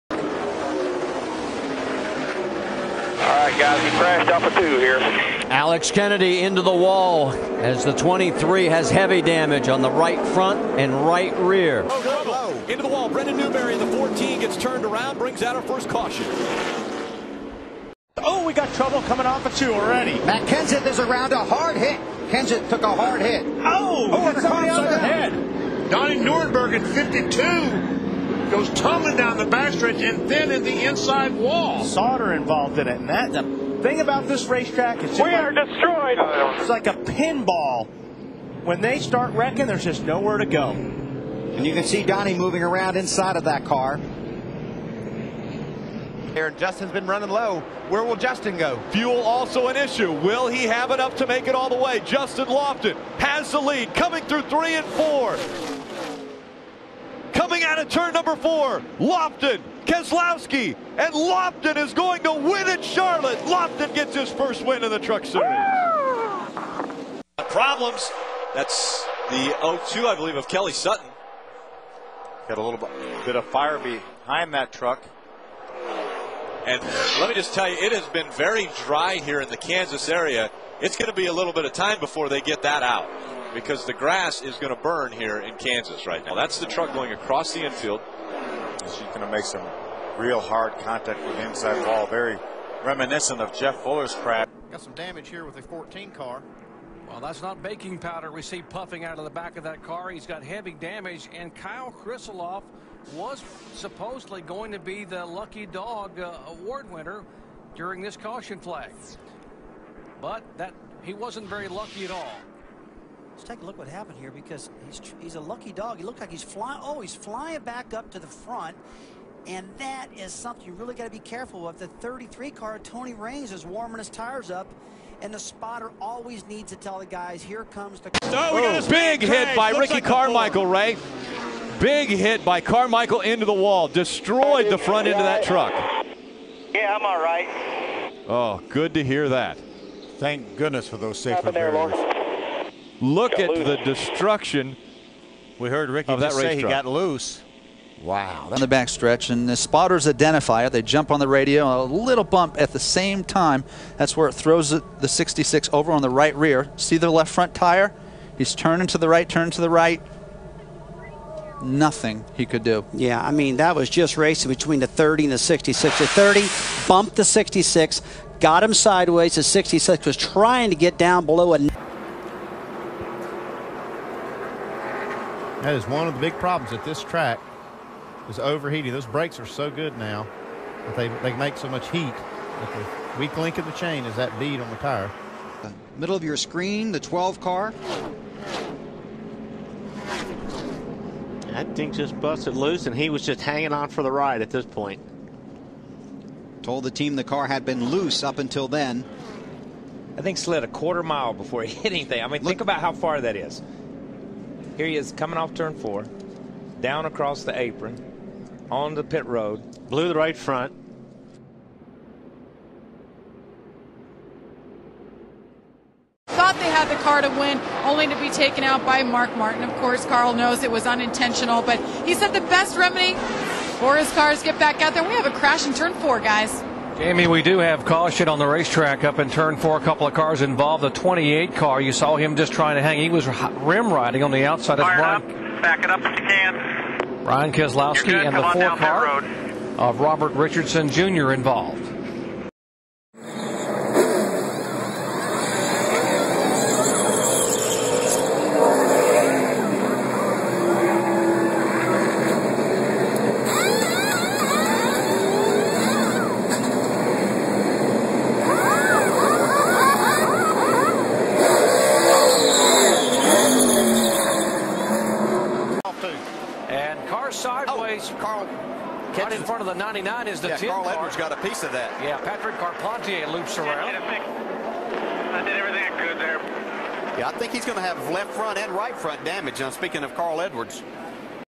Forgotten NASCAR Drivers #3
[4/25] Thanks for 4,300 views!

3:05 is David Stremme, for those who are wondering. Also, there will be multiple more episodes of this series, so don't think it's gonna die any time soon! (Might even reach 10+ episodes at this pace)

List:
Kelly Sutton
Steven Wallace
Brennan Newberry
Donnie Neuenberger
Kyle Krisiloff
Justin Lofton
Ricky Carmichael
Alex Kennedy
Tanner Berryhill
David Stremme
Brian Keselowski
Jason White
Patrick Carpentier